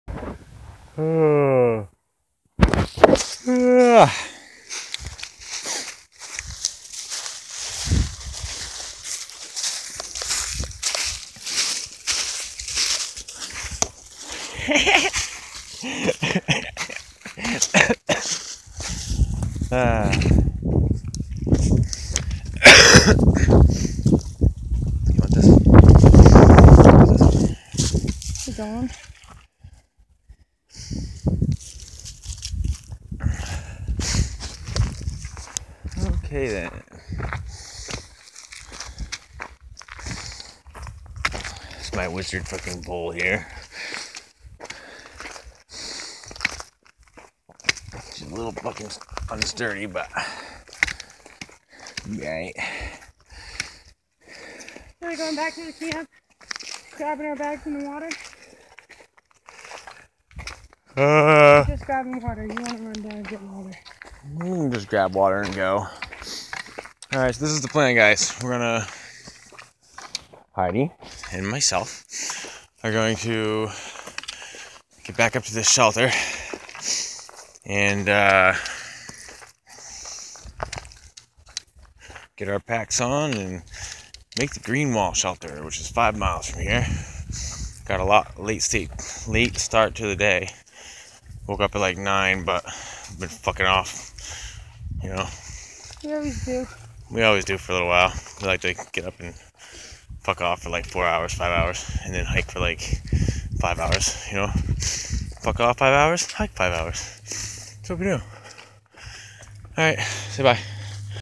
American hiking adventures east coast somewhere out there on the AT
Appalachian trail. Hiking